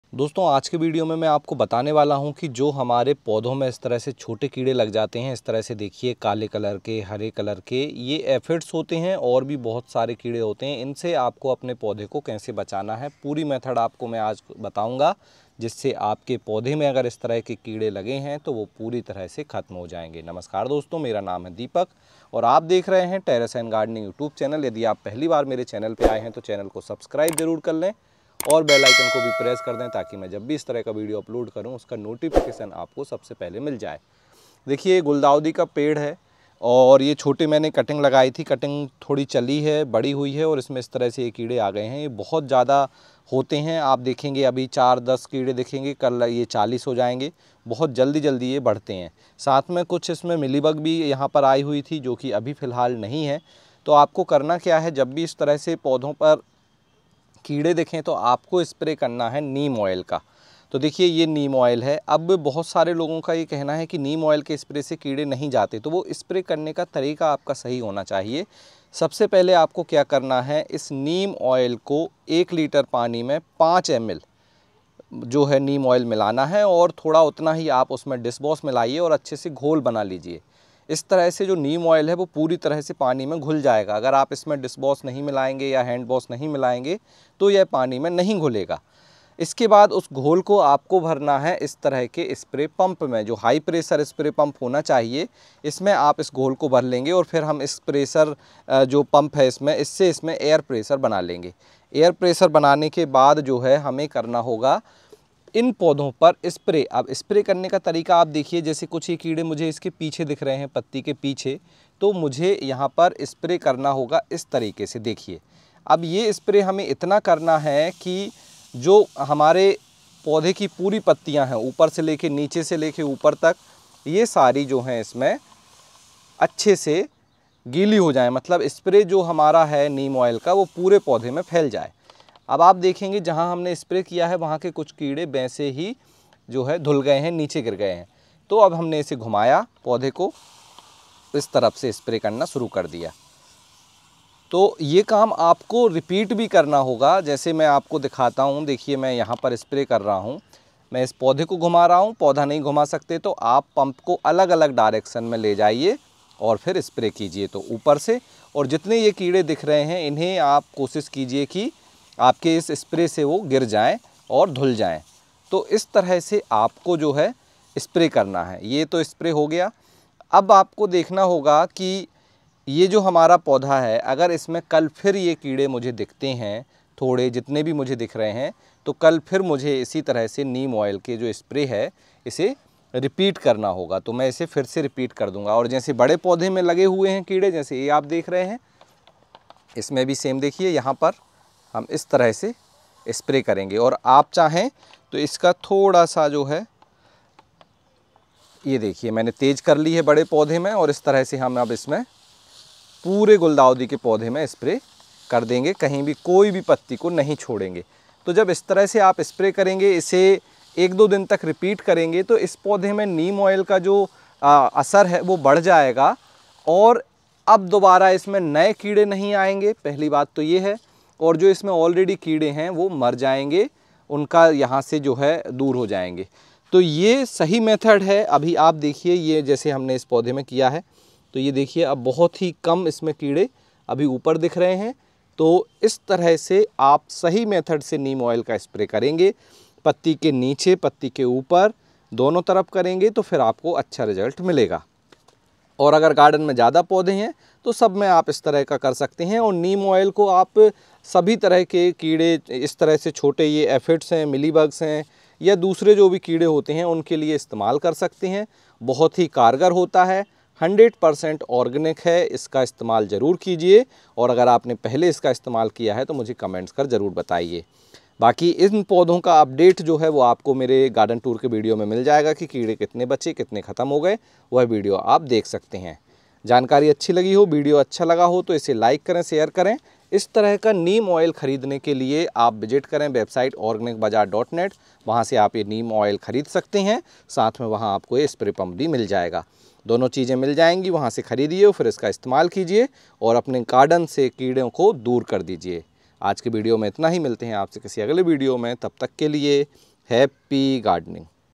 पौधे में कीड़ा लगने पर क्या करें? गमले के पौधों को कीड़ों से कैसे बचाएं? | How To Get Rid Of Insects
घर की छत पर Home Gardening करते हैं और पौधों में लग गए हैं कीड़े तो ये आसान Tips बेहद काम आएगी देखें छोटे छोटे कीड़ों को कैसे नष्ट करें? पौधों को कीट पतंगों से बचाने का सबसे अच्छा तरीका क्या है?
How To Get Rid Of Insects In Plants पौधे में कीड़ा लगने पर क्या करें? गमले के पौधों को कीड़ों से कैसे बचाएं?